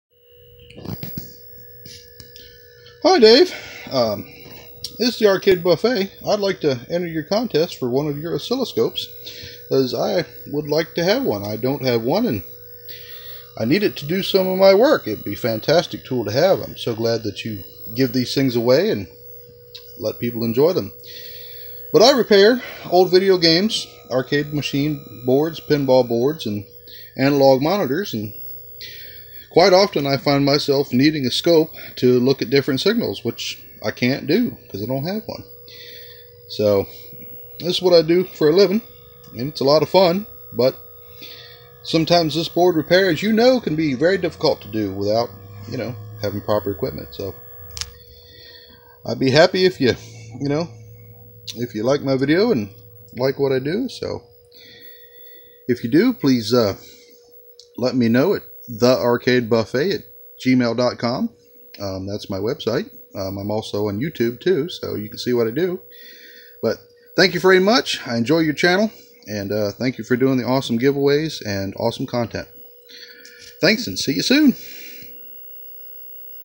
EEVblog Contest video.
HI, i am a self employed fella that enjoys fixing vintage Arcade and Pinball equipment for folks all over the world.

i would be so happy to win a scope as it would help greatly in my repairs.

we all know how taking a shot in the dark can cause damage to old equipment if you are not careful.
this would help troubleshooting go much faster and safer.

thanks for giving theses away to the lucky winners.